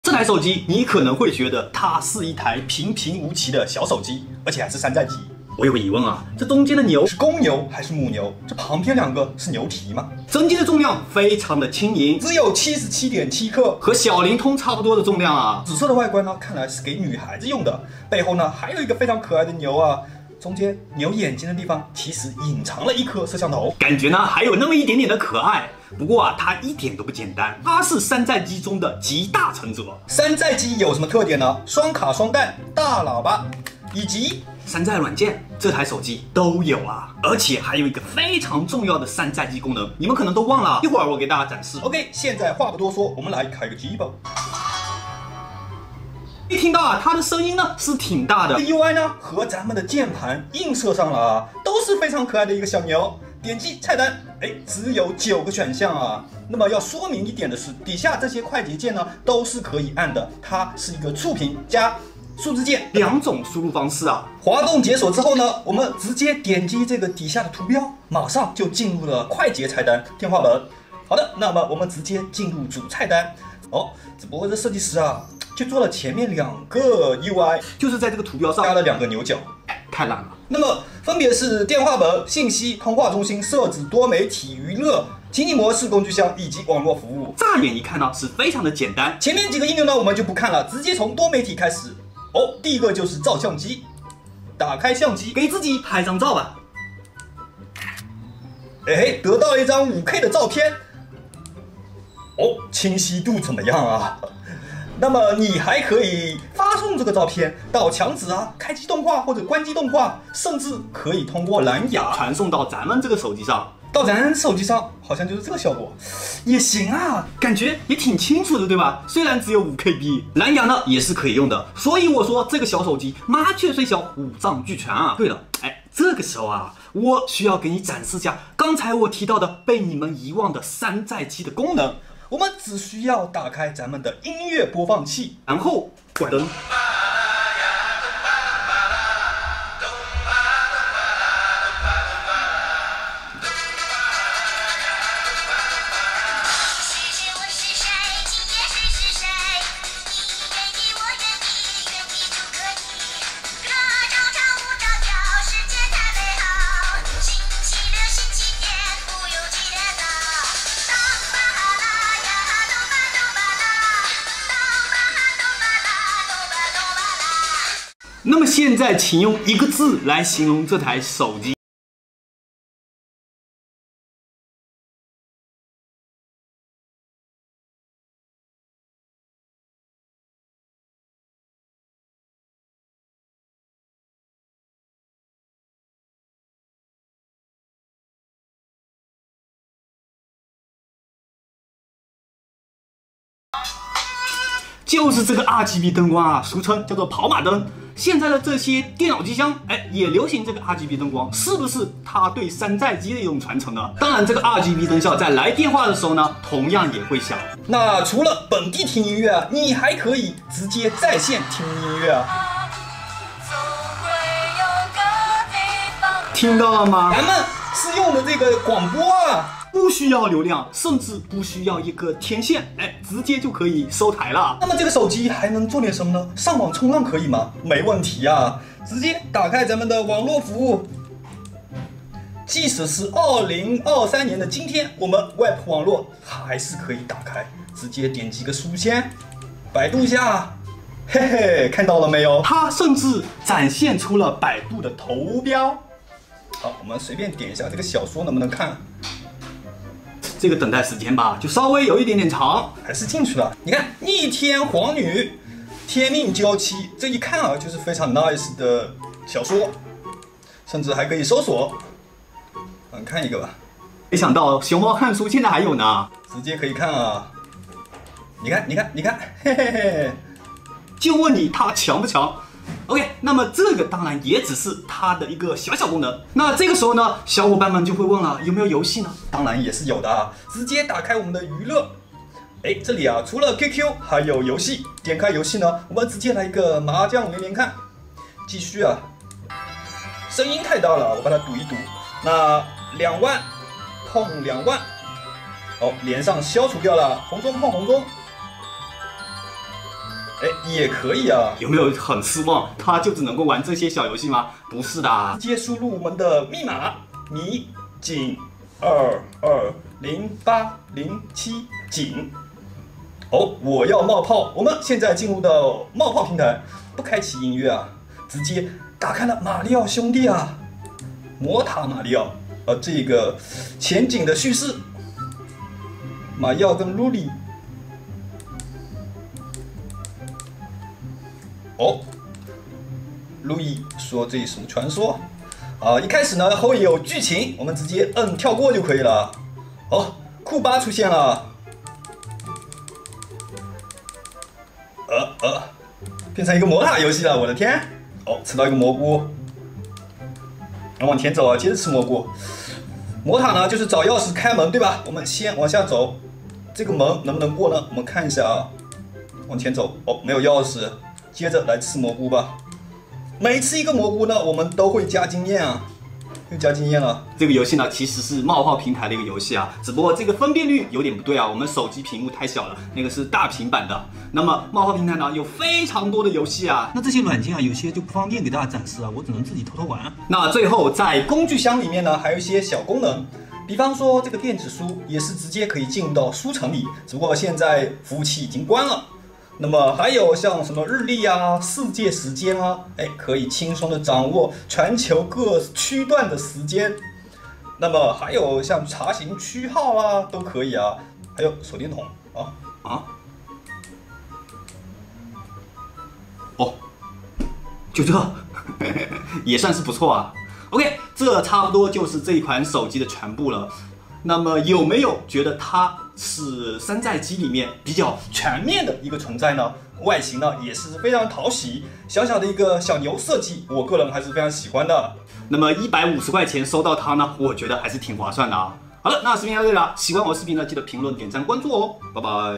150元买到，外形相当可爱功能相当炸裂的山寨机，你一定没见过！
150元买到，外形相当可爱功能相当炸裂的山寨机，你一定没见过！